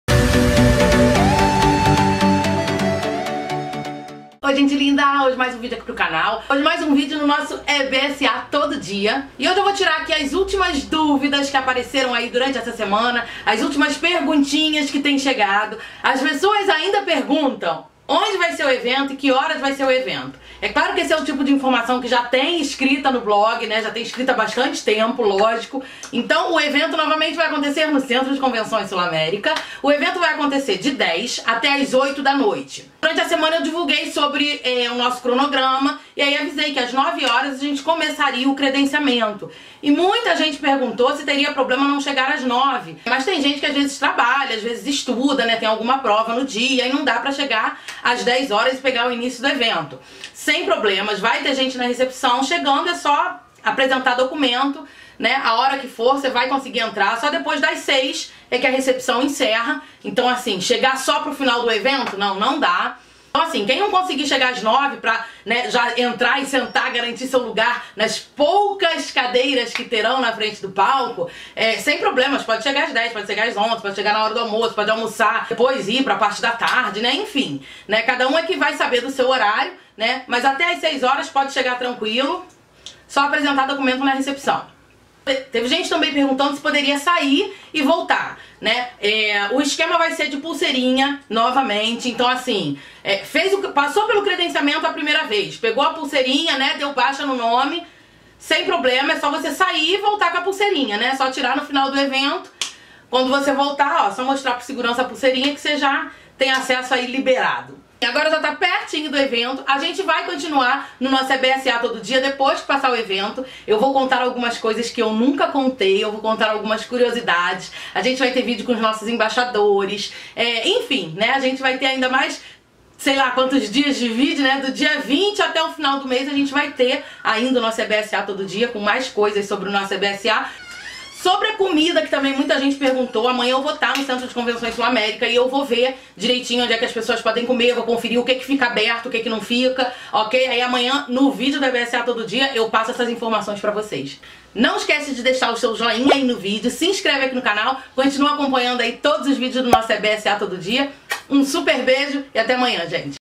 EBSAtodoDia | Qual o melhor horário?
O evento abrirá o credenciamento as 9 horas. Abertura as 10 e encerramento as 20.